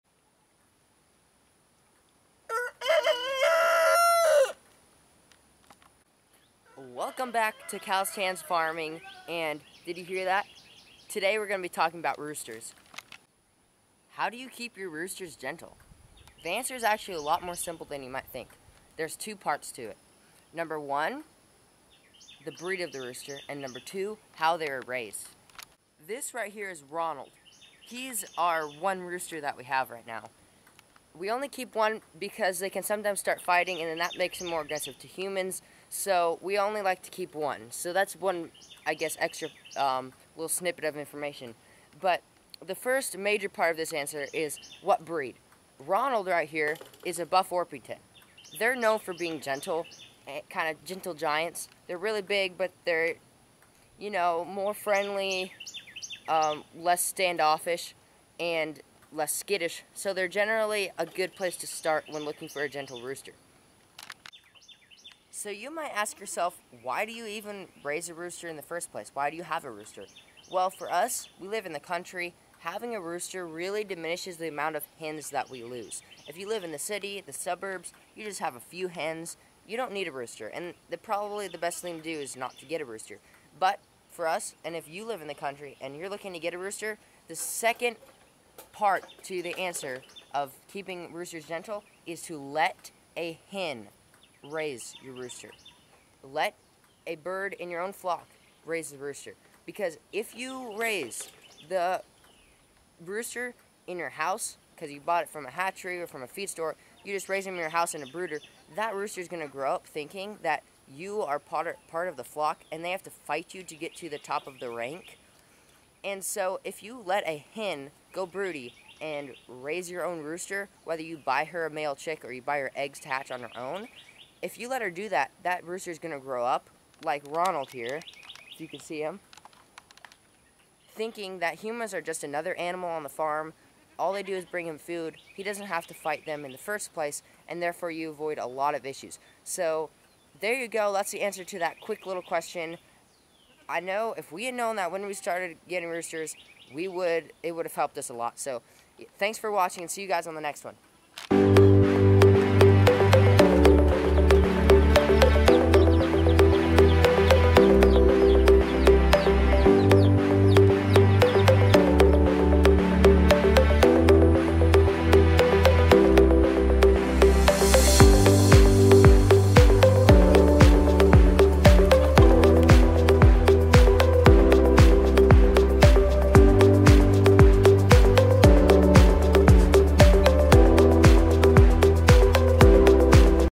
How We Keep ROOSTERS GENTLE!
It is simpler than you might think to have a gentle rooster! Watch this video to find out the answer.